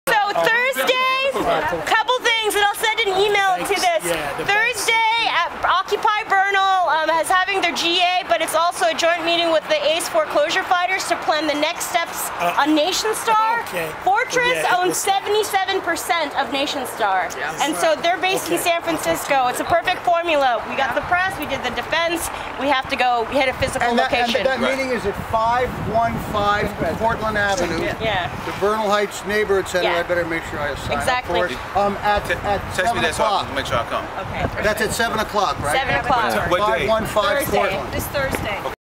Part XII of Kim Mitchell Eviction Defense From Nationstar in San Francisco
Part XII of Kim Mitchell Eviction Defense From Nationstar in San Francisco. Community organizers with the Alliance of Californians for Community Empowerment (ACCE), Occupy Bernal, Occupy Noe, Occupy SF Direct Action Workgroup, and supporters and neighbors gathered starting at 6:00am on August 22, 2012, succeeded in pressuring Nationstar to postpone the eviction of Kim Mitchell and his family from the San Francisco home. Next, the Mitchell family hopes to negotiate with Nationstar to obtain a fair deal loan modification so they can remain in their home. (MVI_0052.AVI)